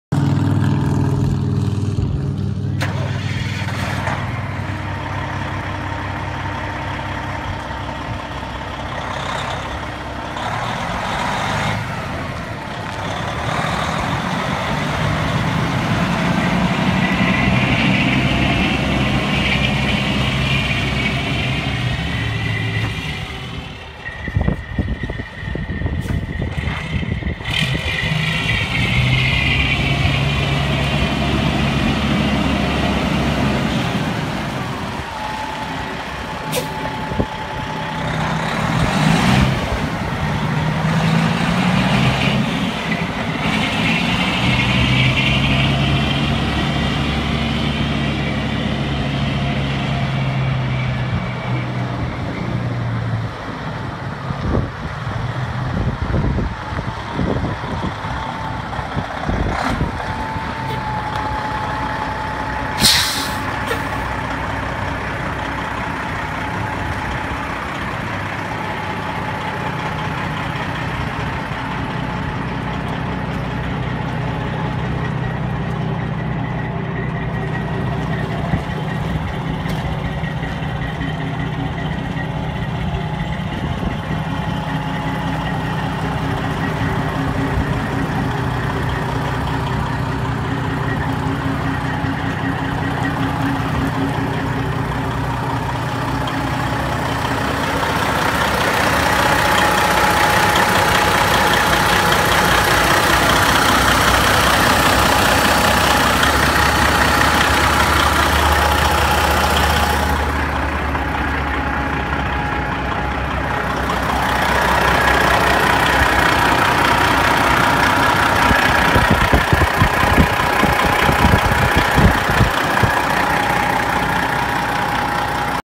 Selling on BigIron Online Auctions 7/27/2016 CW9726
1999 Volvo semi tractor, 430/500 Detroit, 13 speed, mid-roof sleeper, near new rubber, needs turbo, yard drives